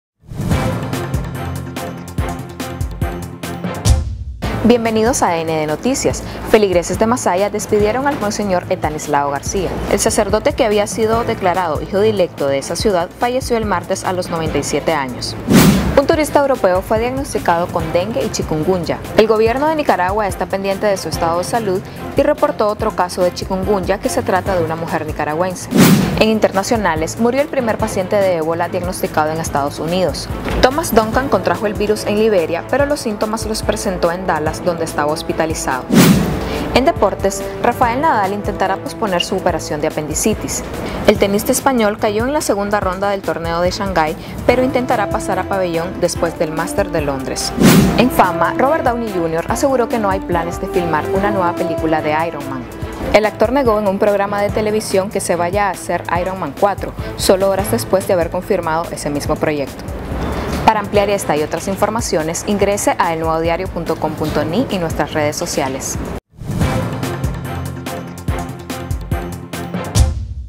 END Noticias: Muere paciente de ébola en EEUU
En nuestro micronoticiero de hoy presentamos: turista europeo fue diagnosticado de dengue y chikungunya, Robert Downey Jr. desmiente que se vaya a hacer Iron Man 4. Esto y más en 60 segundos.